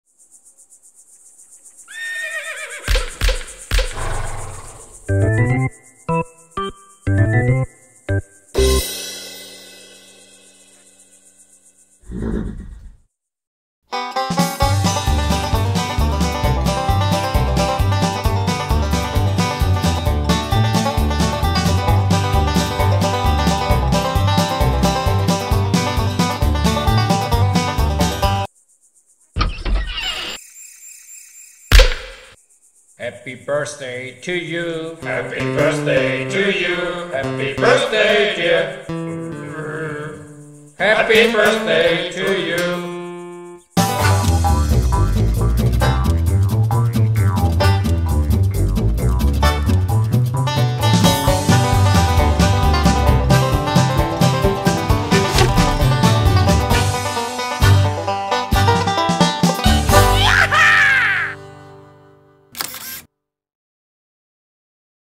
Zum Geburtstag (Wilderwesten)
Happy Birthday! Zum Geburtstag an alle registrierten Kunden lustige Geburtstagsgrüße versenden, das war die Idee hinter der mehrjährigen Online Marketing Maßnahme für die eCommerce Plattform Quelle (†2009). Konzept, Idee und Umsetzung sind von der Werbeagentur 2hoch11 aus Nürnberg. Drei weitere Filme wurden erstellt, die auch im 2hoch11 Channel zu finden sind.
YEEE-HAAA !